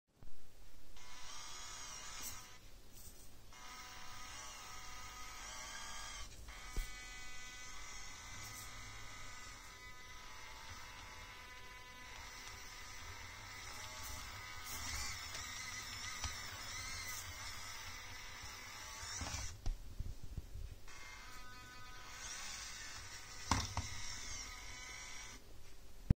Orlandoo Hunter OH32A03 D110 Defender custom - Tools crawling - Just do it .
Servo from chassis to axle.
Upgrade 2.2g stock servo with big 3.7g servo.
Some upgrade ...